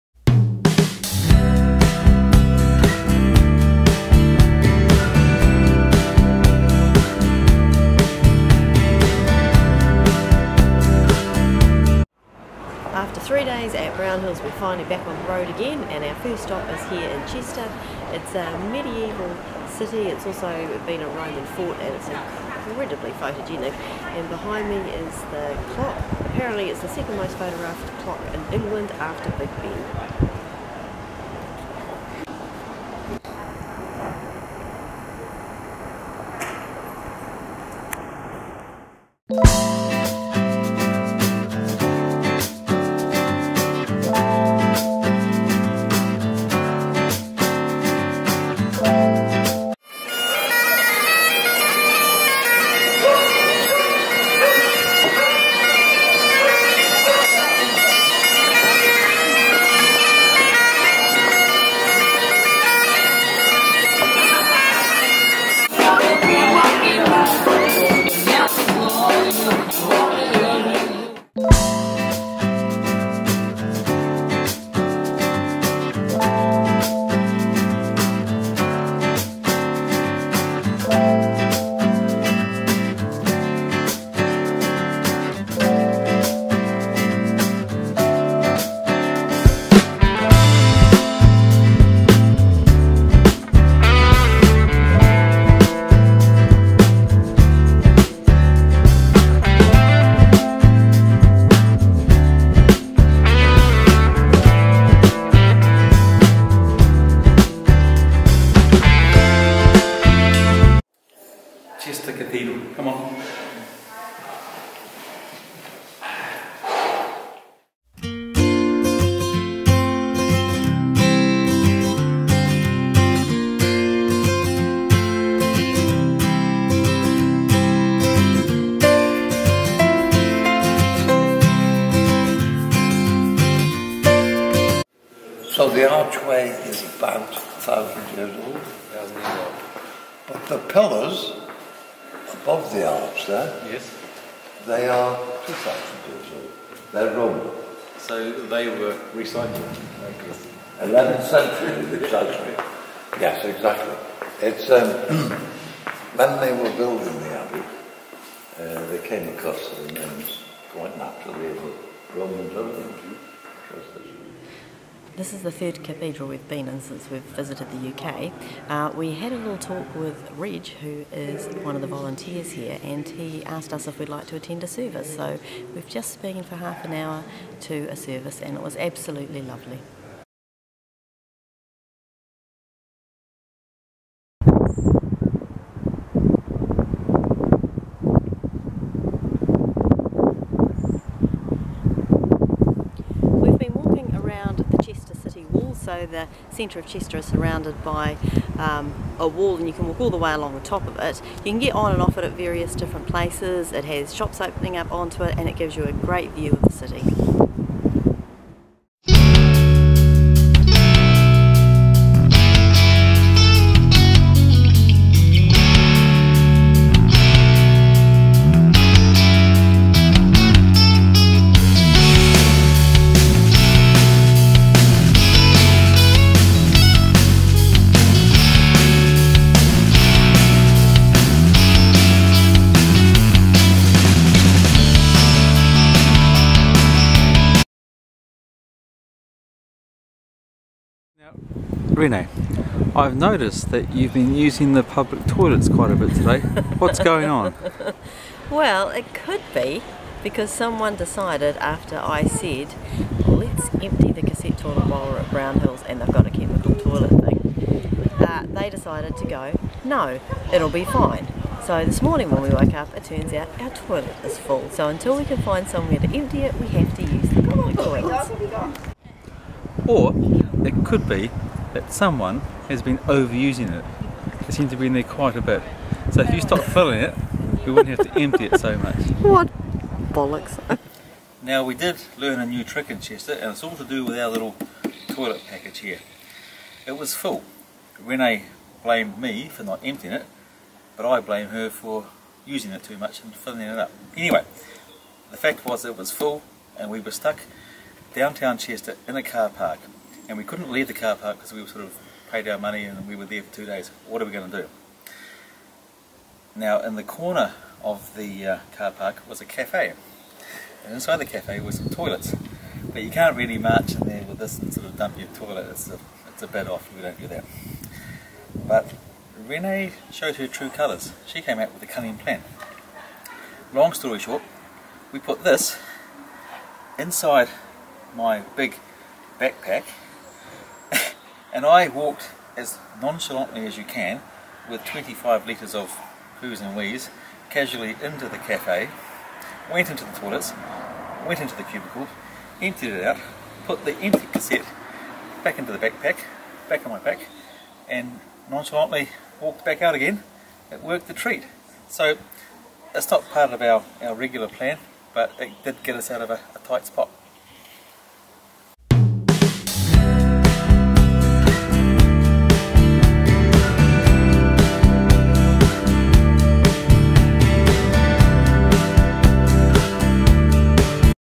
CHESTER
A quick trip to the beautiful city of Chester. And the not so glamorous side of motorhoming!

A couple of middle-aged Kiwis travelling the world by plane, train and motorhome. The kids left home and so did we!